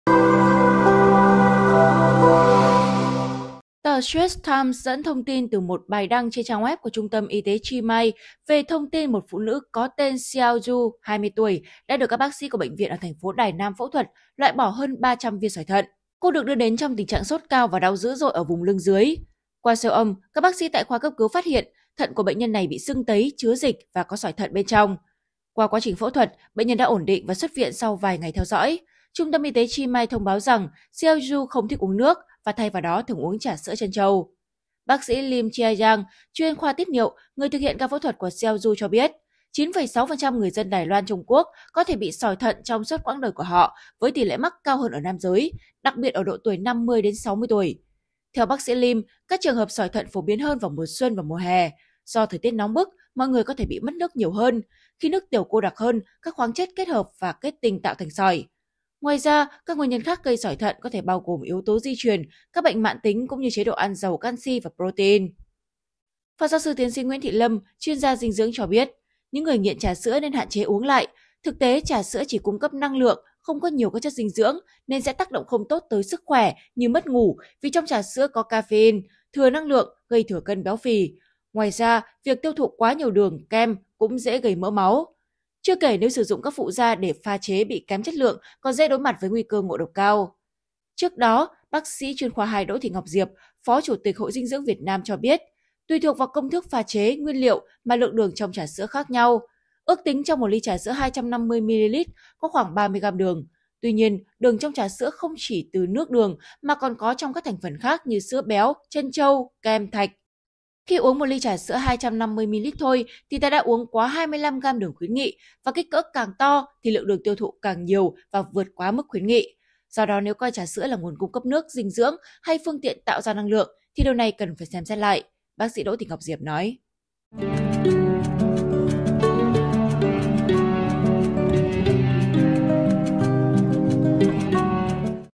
Lấy Ra Hơn 300 Viên Sỏi Thận Trong Người Vì Thói Quen Uống Trà Sữa Thay Nước | SKĐS
SKĐS | Một người phụ nữ 20 tuổi ở Đài Loan đã phải loại bỏ hơn 300 viên sỏi thận bởi thói quen uống trà sữa trân châu thay nước của mình.